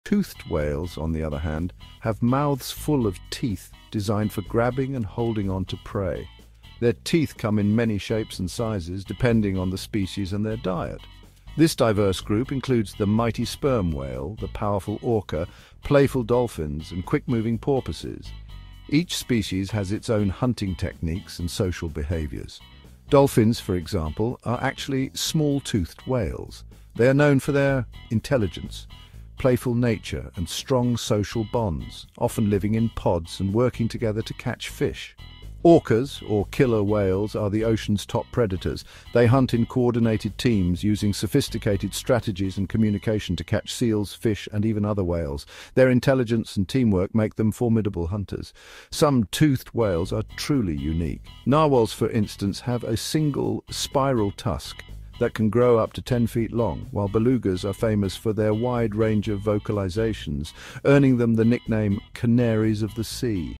Toothed Whales: Orcas, Dolphins & Amazing Marine Life
Toothed whales boast mouths full of diverse teeth, perfectly adapted for grabbing prey. From mighty sperm whales to playful dolphins, discover their unique hunting techniques and complex social lives. Witness the ocean's apex predators in action!